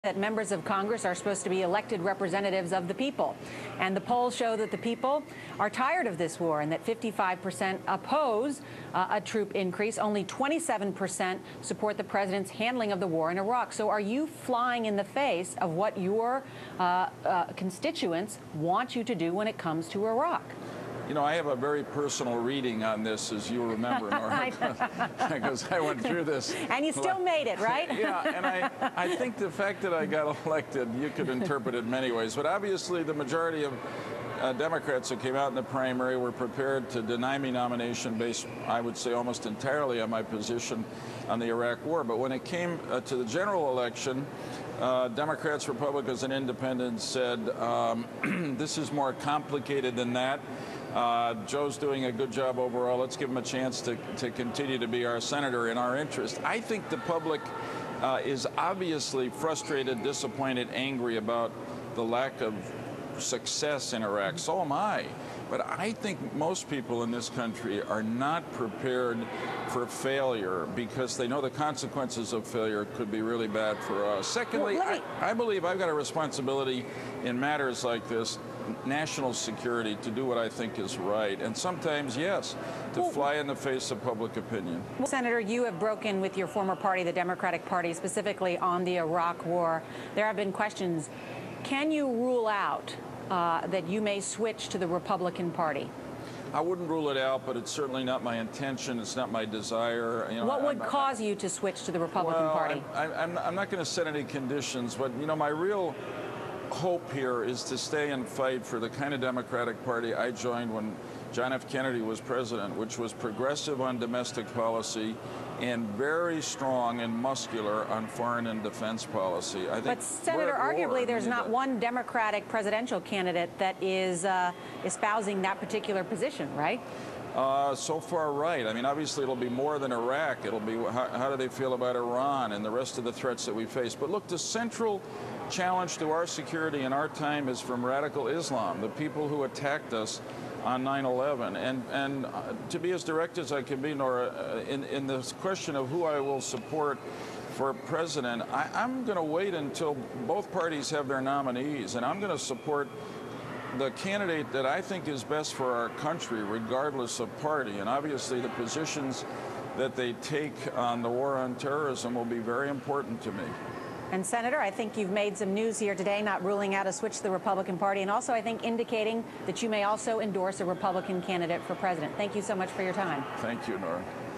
Lieberman spitting in the public's face/ plans to jump ship
From MSNBC, heres Joe Lieberman spitting in the public's face and making plans to jump ship to the Republican Party